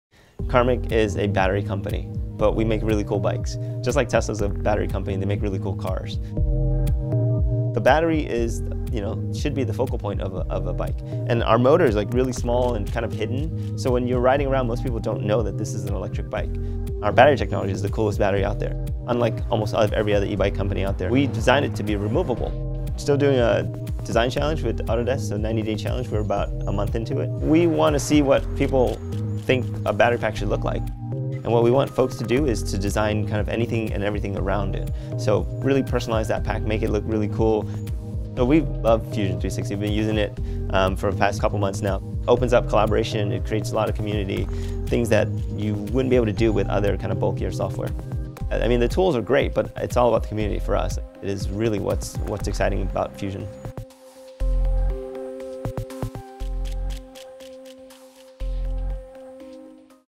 Karmic Bikes Koben Overview
Watch the Karmic Koben in action, and Hong Quan talk about the philosophy of the Karmic electric bike and how they use Fusion 360.